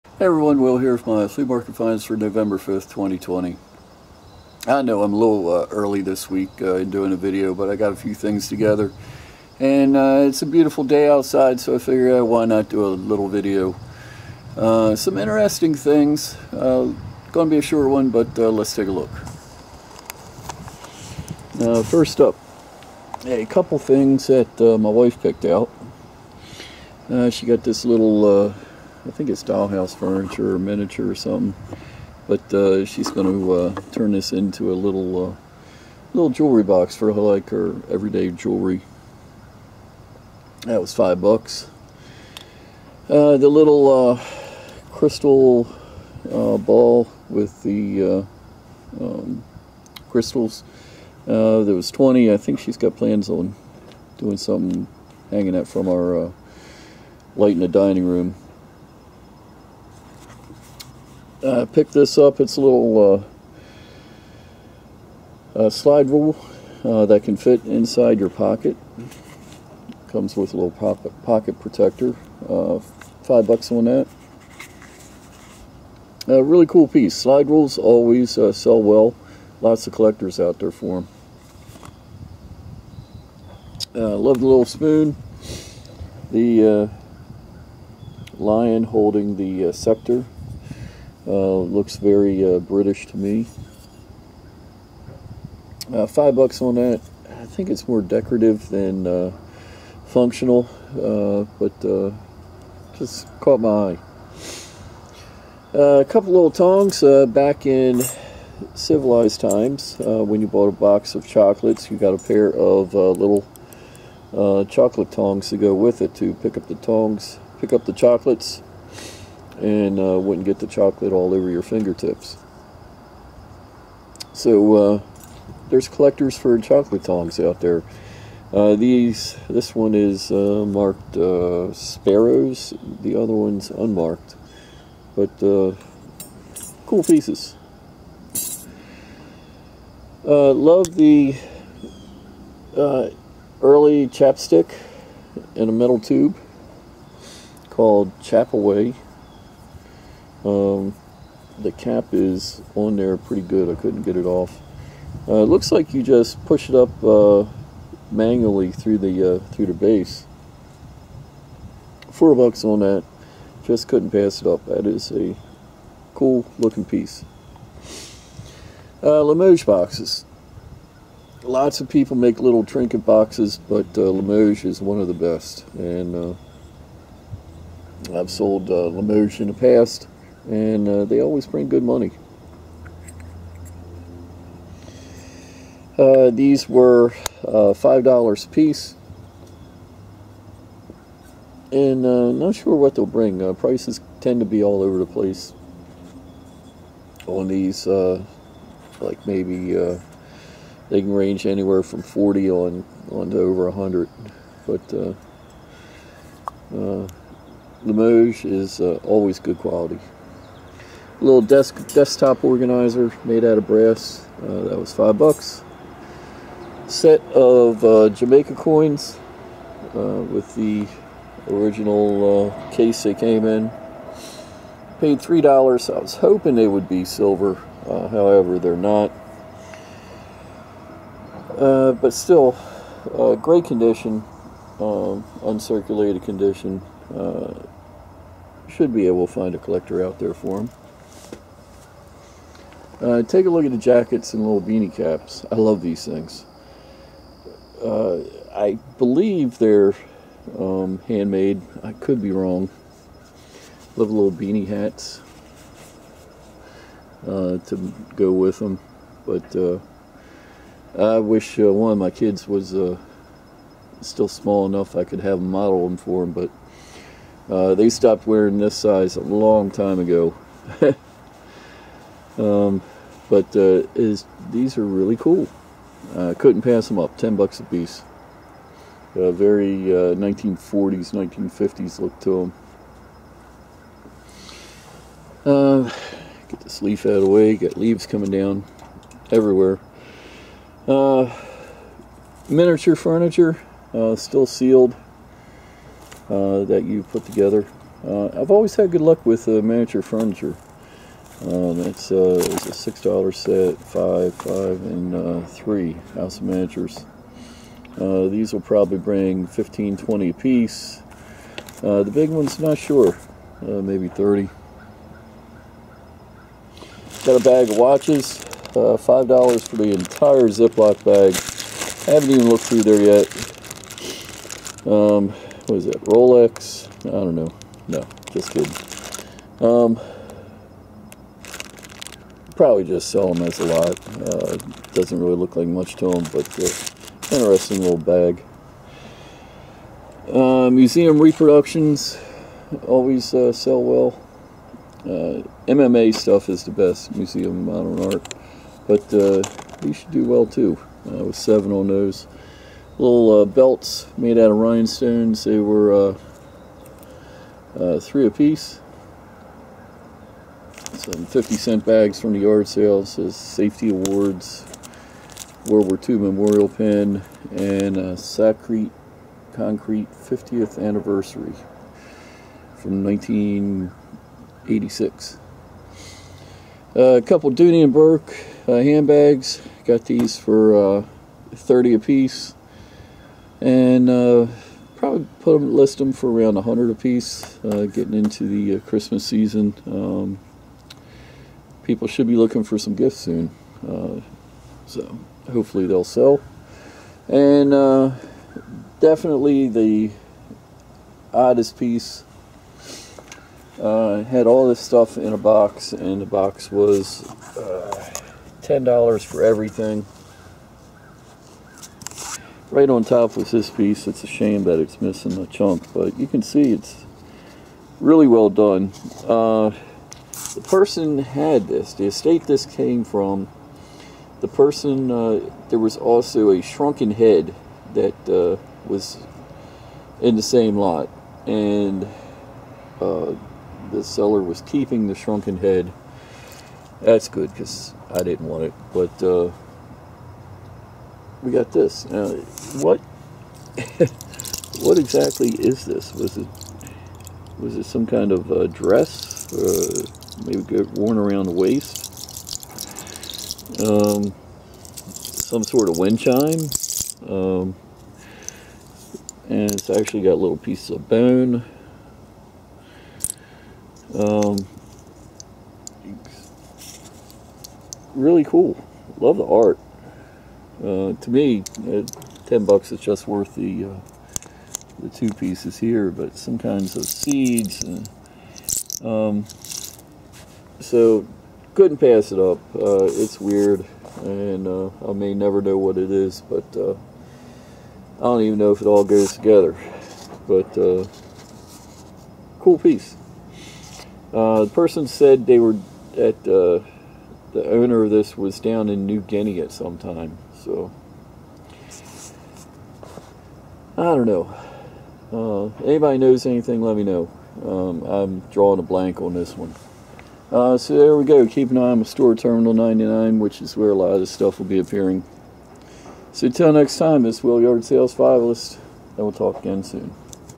Mini Estate Sale Picking Finds Video
A few cool looking items and one weird thing are in this mid week video.
My eBay jewelry store;
My main eBay store;